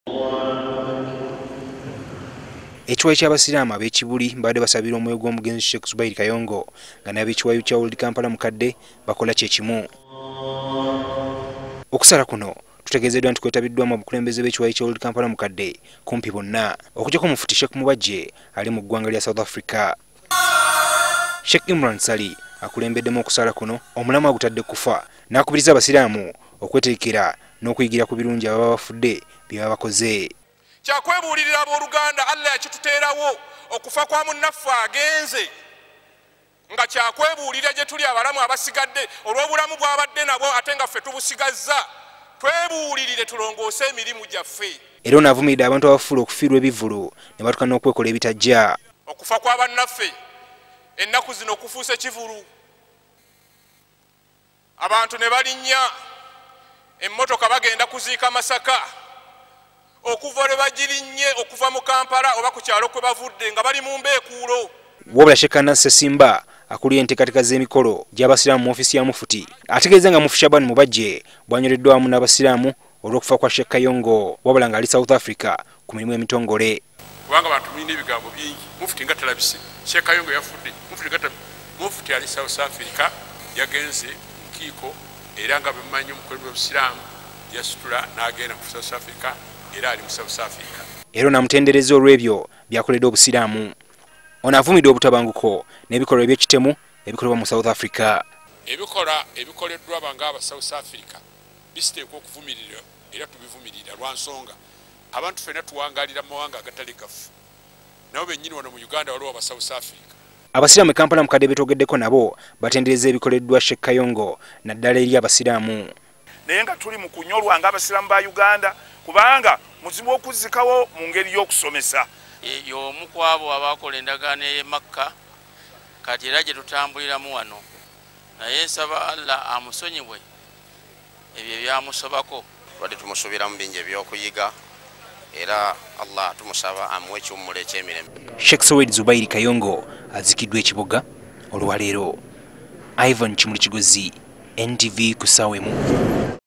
Ab’e Kampala mukadde basabidde Sheikh Kayongo
Ku muzigiti omukulu e Kampala mukadde nayo wabaddeyo okusabira Mufti Kayongo , mukama gweyajjuludde mu bulamu bwensi eno . Bano bamutendereza olwebyo byakoledde obusiraamu.